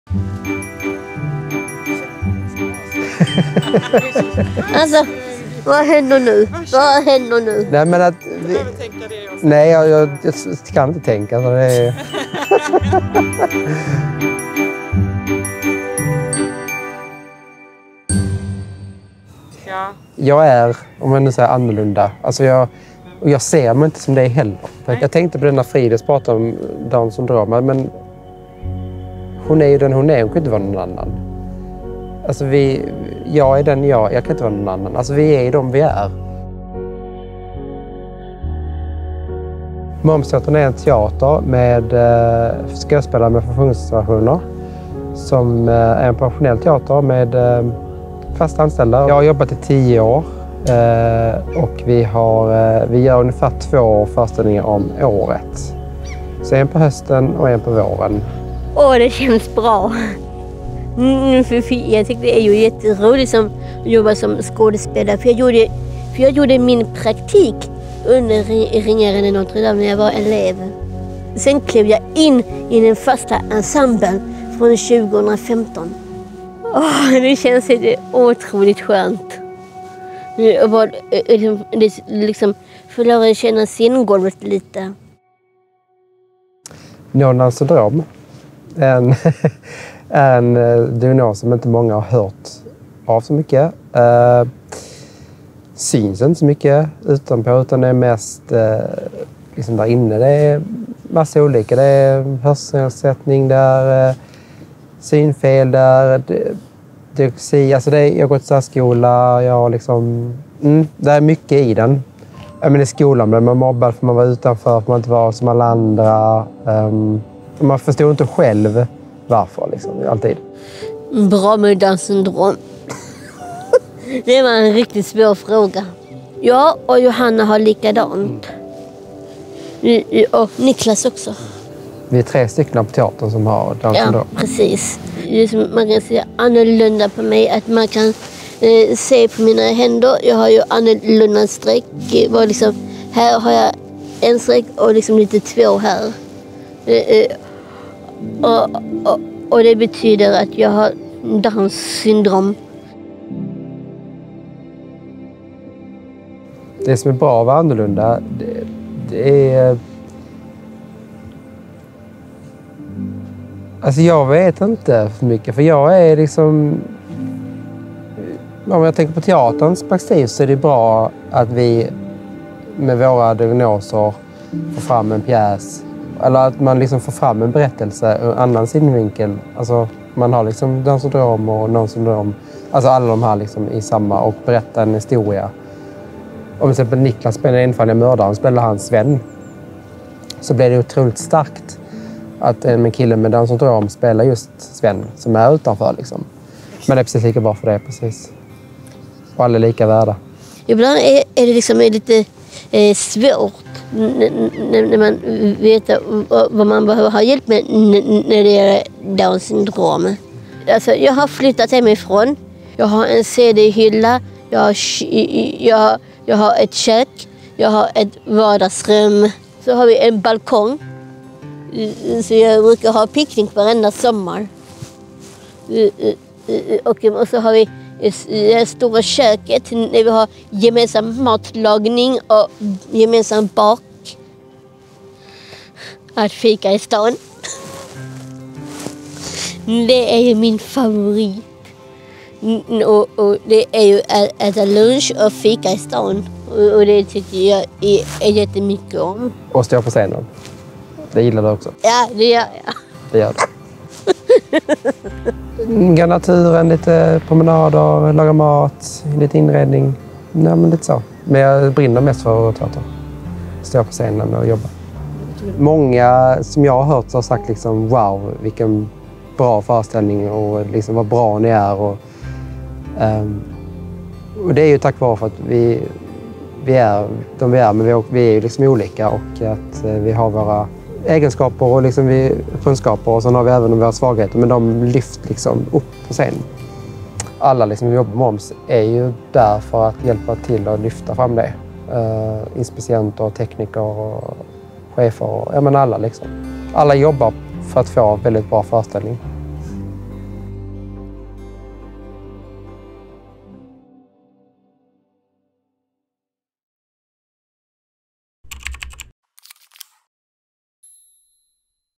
Moomsteatern – när funktionsvariationer lyfter scenkonsten
”Vi är dem vi är! Vi kan inte vara någon annan.”

Alla människor har en unik uppsättning av förmågor och sätt att fungera. Om man som skådespelarna Dennis Mortensson och Frida "Fridis" Schriver, har en funktionsvariation, betyder det att man på olika vis fungerar på sätt, som avviker från normen; fysiskt och/eller psykiskt och kognitivt.

Moomsteatern i Malmö är känd för sina innovativa, varma och proffsiga teateruppsättningar. De skapar normbrytande scenkonst och utmanar teaterkonsten tillsammans med skådespelare med eller utan olika typer av funktionsvariationer. Alla skådespelare är anställda och har möjlighet att fokusera och utvecklas inom skådespelaryrket.

Dennis har varit anställd på teatern i tio år och brinner för yrket. Fridis började som praktikant, ingår nu i den fasta ensemblen och älskar att stå på scen.

Fridis har Downs syndrom, en funktionsvariation som många känner till. Sjukdomen innebär att man föds med en extra kromosom. Detta påverkar hjärnans utveckling och gör att de allra flesta barn med Downs syndrom får en intellektuell funktionsnedsättning, oftast en lindrig sådan. De flesta lär sig läsa och skriva i särskolan och kan ha ett enklare jobb i vuxen ålder. Många gånger behövs hjälp med planering och ekonomi.

Dennis har ett mer ovanligt syndrom som upptäcks redan vid födseln; Noonans syndrom. Sjukdomen ger många olika symptom som kan variera från person till person. I Dennis fall finns ingen intellektuell nedsättning, men dyslexi, svårt med matte, nedsatt syn och hörsel, är några av anledningarna till att han fick gå i särskola.

Personer med Downs eller Noonans syndrom, eller andra funktionsnedsättningar är självklart först och främst människor med lika unika förmågor, samma grundläggande behov och värde som alla andra individer. Vissa behöver lite mer hjälp bara.